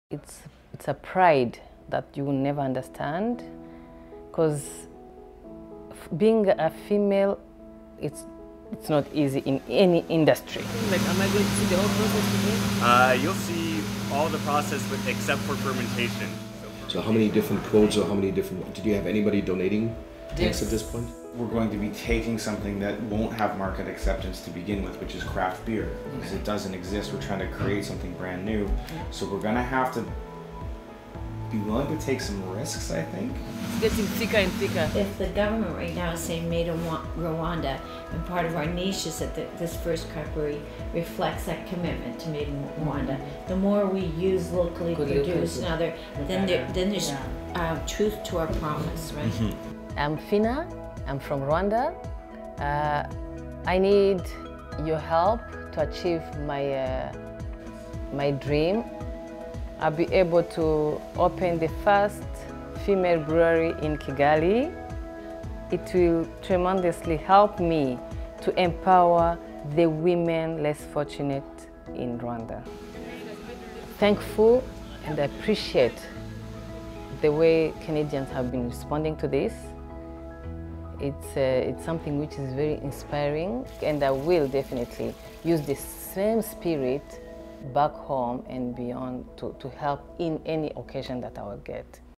The Rwanda Craft Brewery Project - Update
The first two weeks of this Kickstarter have been an exciting time! We've received incredible support from our backers, and interest from many people in the craft beer industry, those interested in learning more about the project, and some wonderful media coverage as well. Thank you for your support and interest.